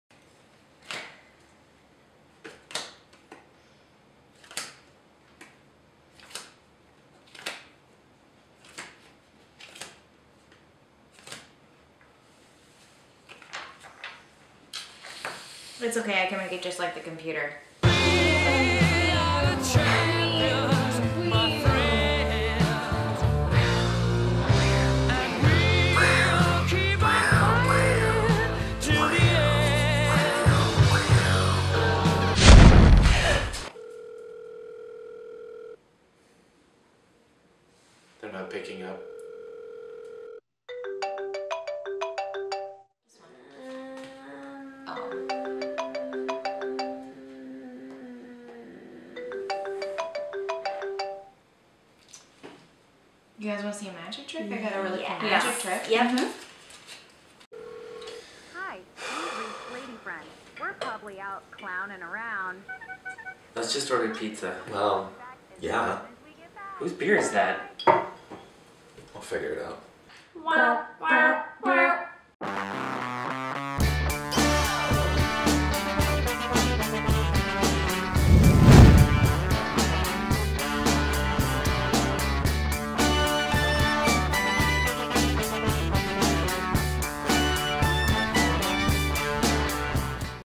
Ladyfriend Wins at Solitaire
Ladyfriend is too busy perfecting their Windows 98 impression to pay attention to Tim, Troy & Damian. Yeah, that's it... too busy. Soooo busy.

Promo video for Ladyfriend + The Adventures of Tim, Troy & Damian.